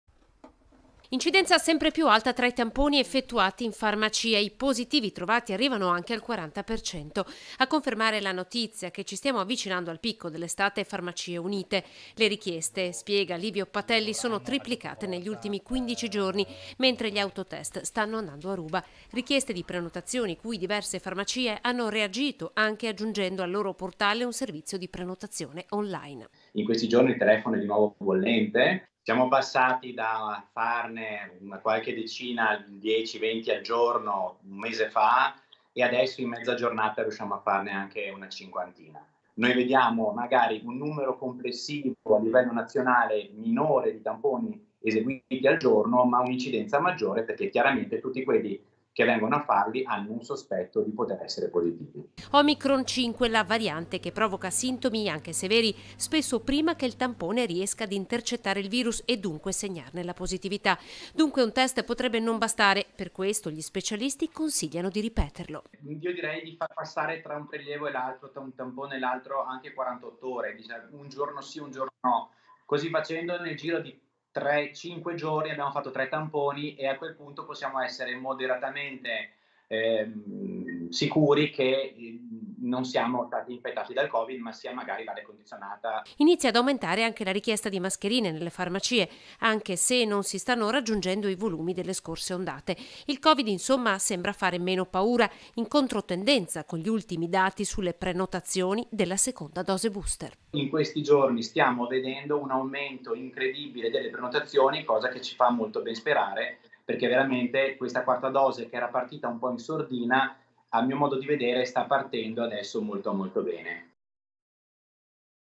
BOOM DI TAMPONI NELLE FARMACIE E INCIDENZA SEMPRE PIU' ALTA | 14/07/2022
A3 NEWS Treviso 14/07/2022 - TREVISO - Incidenza dei positivi nei tamponi effettuati in farmacia semrpe più alta, Farmacieunite parla di un 30-40% . Buoni i dati sulle prenotazioni per la quarta dose.   || Incidenza sempre più alta tra i tamponi effettuati in farmacia: i positivi trovati arrivano anche al 40%. a confermare la notizia che ci stiamo avvicinando al picco dell'estate Farmaciunite: Le richieste sono triplicate negli ultimi 15 giorni mentre gli autotes stanno andando a ruba. Richieste di prenotazioni cui diverse farmacie hanno reagito anche aggiungendo sul loro portale un servizio di prenotazione on line.Omicron 5, la variante che provoca sintomi anche severi spesso prima che il tampone riesca ad intercettare il virus e dunque segnare la positività. Dunque un test potrebbe non bastare, per questo gli specialisti consigliano di ripeterlo. Inizia ad aumentare anche la richiesta di mascherine nelle farmacie anche se, conferma Livio Patelli, non si stanno raggiungendo i volumi delle scorse ondate. Il covid, insomma, fa neno paura. In controtendenza con gli ultimi dati sulle prenotazioni della seconda dose booster. - Intervistati LIVIO PATELLI (CONSIGLIERE FARMACIEUNITE) (Servizio di Anna De Roberto) - Segui Antenna Tre anche sul digitale terrestre! Visita il sito